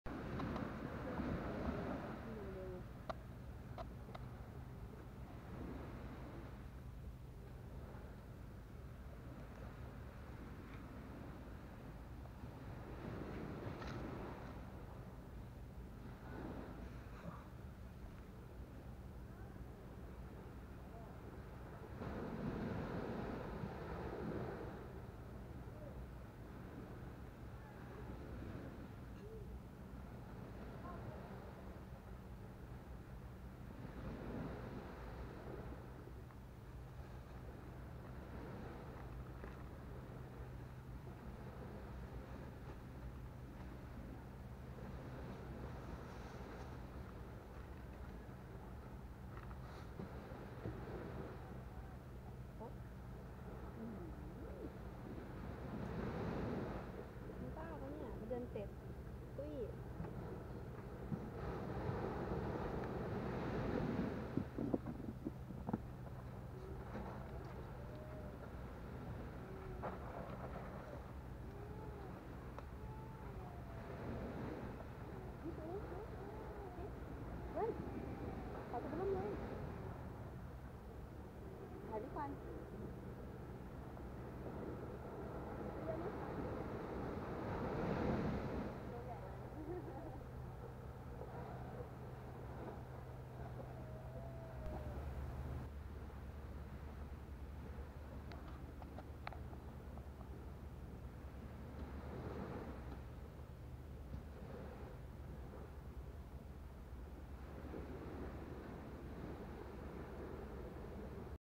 ตุ๊ดตู่... มดมาแล้ว
ตุ้ม แมงช้าง แมลงช้าง หรือแถวๆบ้านผมเรียก ตุ๊ดตู่ จะอยู่ตามพื้นทรายแถวๆชายหาดมีเยอะ ตอนเด็กๆชอบเอามดแดงไปใส่ในหลุมที่มันขุดไว้ บางทีมดก็ขึ้นได้ บางทีก็ขึ้นไม่ได้ ด้วยความสงสัยของเด็ก บางทีก็ขุดมันขึ้นมาดูบ้าง